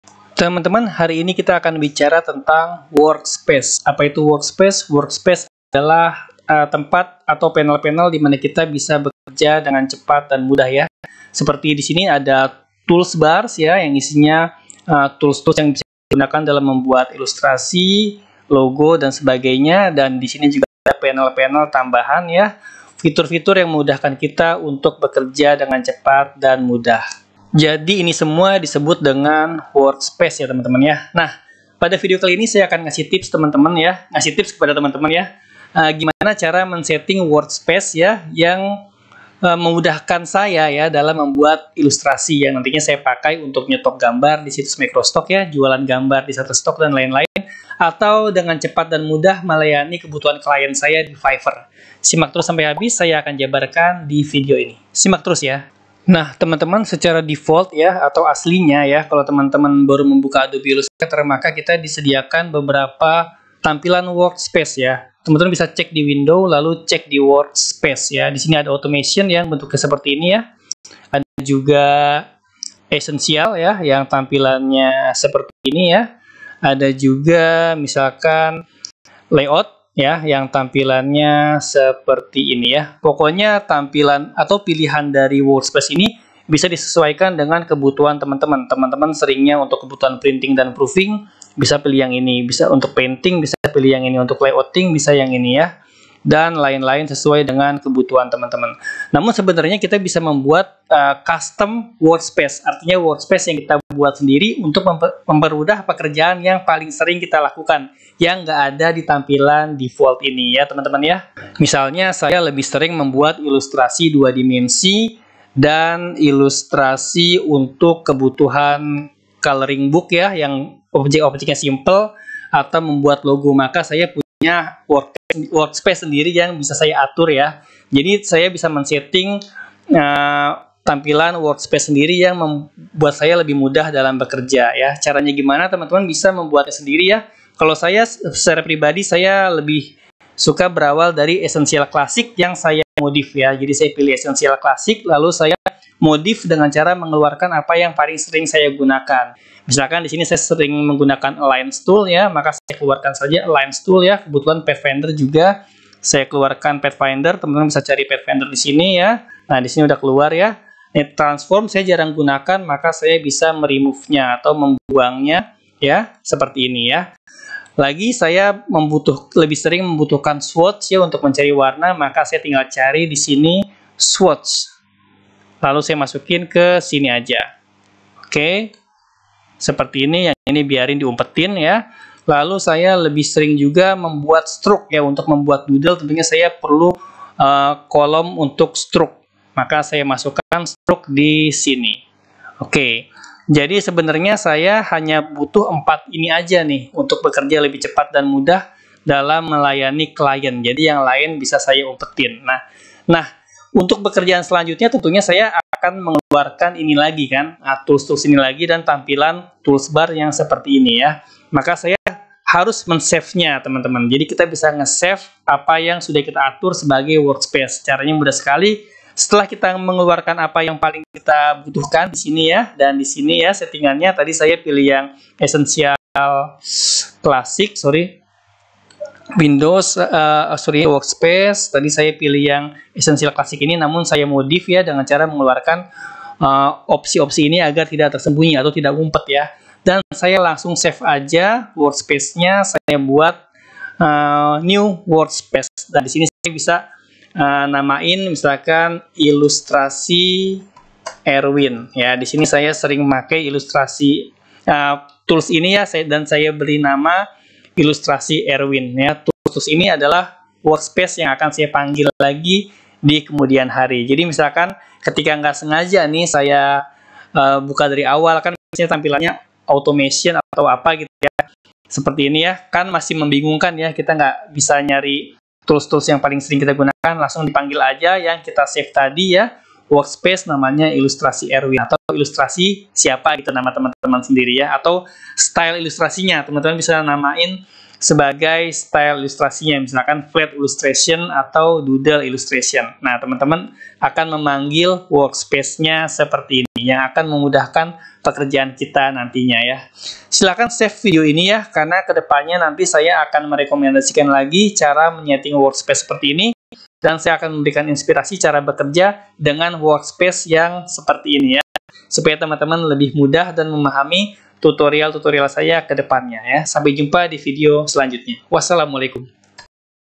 Mengatur Workspace Adobe Illustrator untuk Desain Lebih Mudah | Tips Shutterstock Contributor
Dalam video ini, saya akan membagikan beberapa tips untuk mengatur workspace Adobe Illustrator agar lebih efisien dan memudahkan proses desain. Saya juga akan membahas fitur-fitur penting dalam aplikasi ini yang akan membantu Kamu mengoptimalkan penggunaan workspace Kamu. Jadi, jika Kamu seorang contributor Shutterstock yang ingin meningkatkan kemampuan desain Kamu, pastikan untuk menonton video ini sampai selesai!